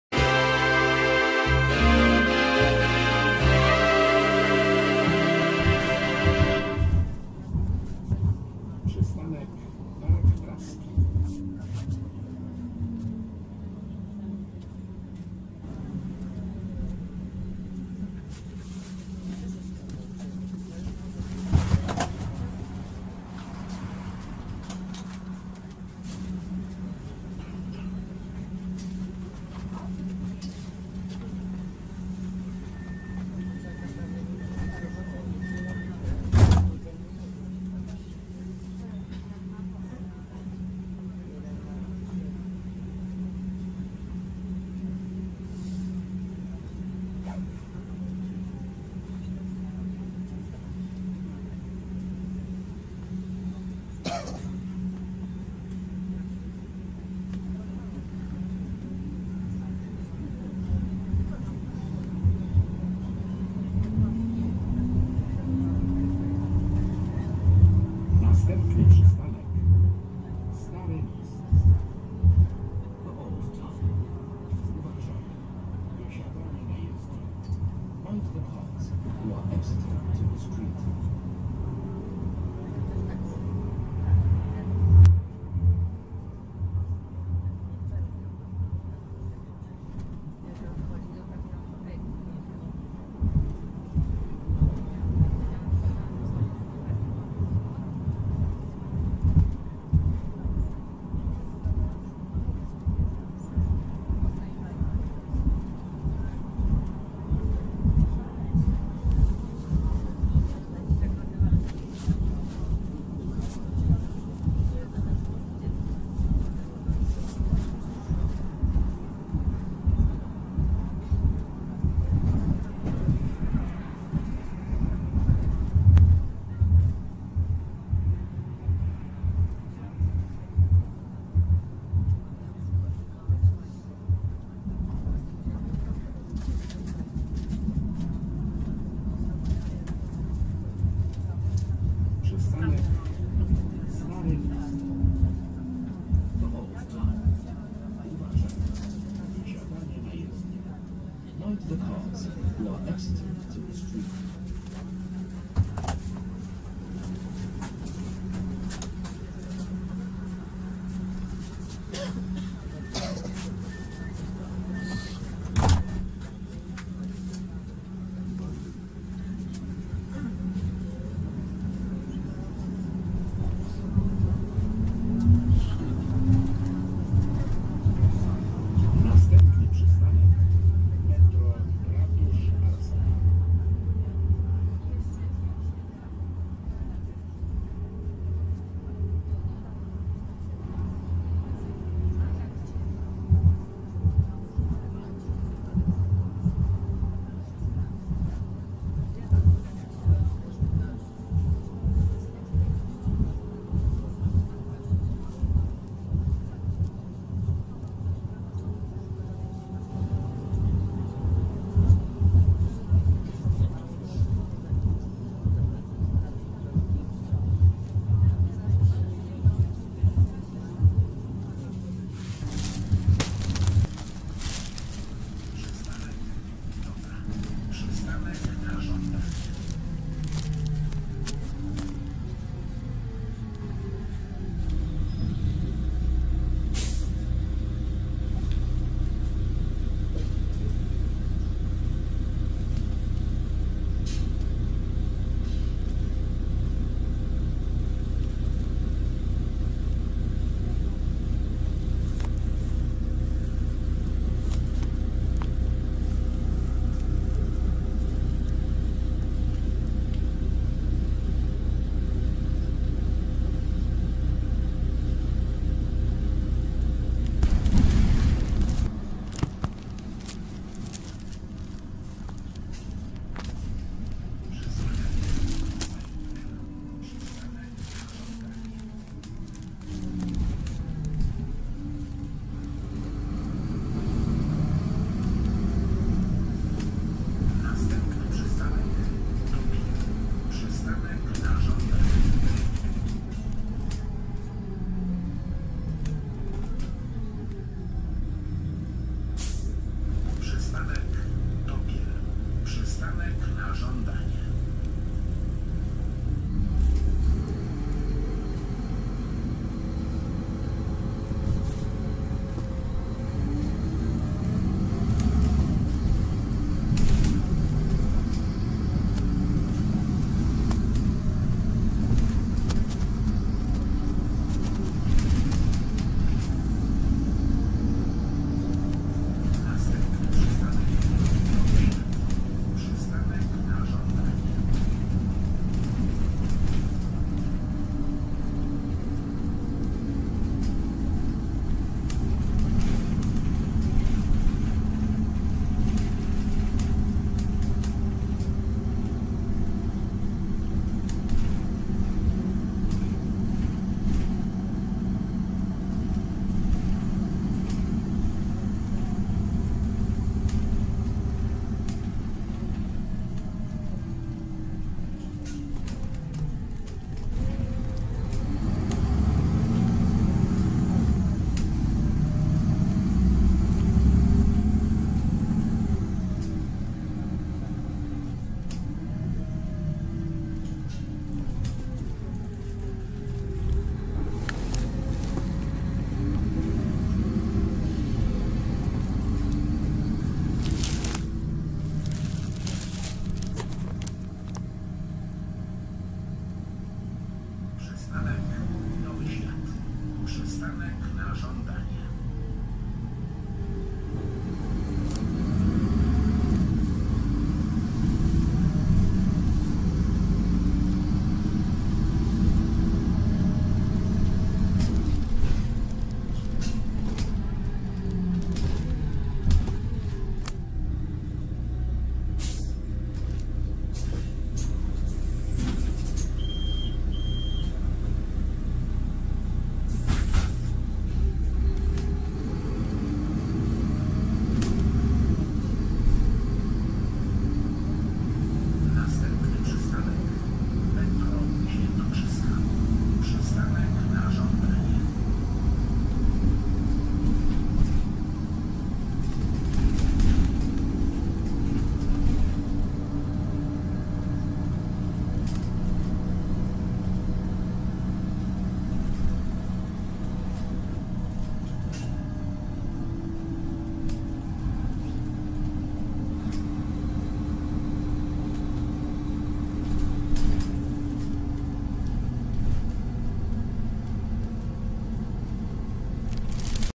Варшавские трамваи Tramwaje warszawskie Warsaw trams
Варшавскиетрамваи Tramwajewarszawskie Warsawtrams Jeden z mozliwych sposobow podrozowania Warszawe -warszawskie tramwaje;szybki,korzystny i tani. Один из возможных способов передвижения по Варшаве – варшавские трамваи, быстро, выгодно и дешево One of the possible ways to travel in Warsaw - Warsaw trams; fast, favorable and cheap.